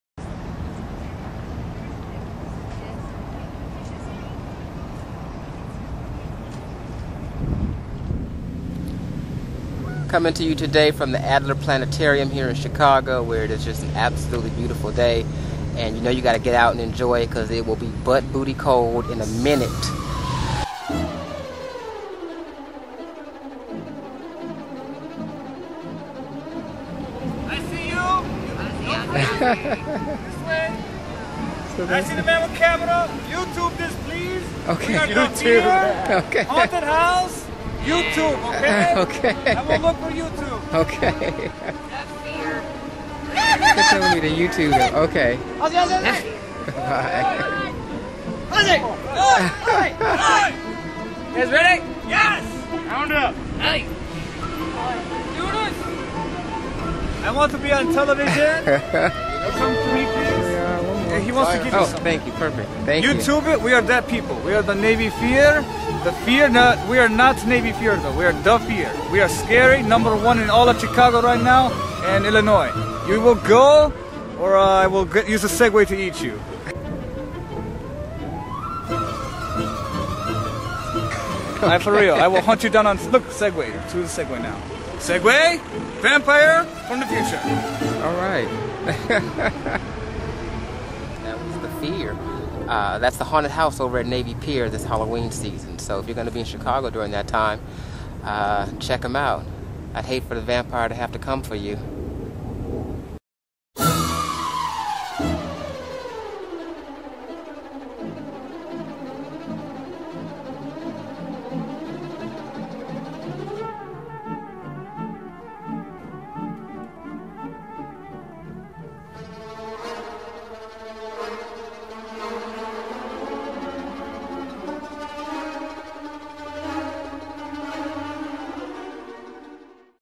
THE FEAR HAUNTED HOUSE AT NAVY PIER
While visiting Chicago's Adler Planetarium I encountered THE FEAR, promoting their haunted house on Chicago's Navy Pier!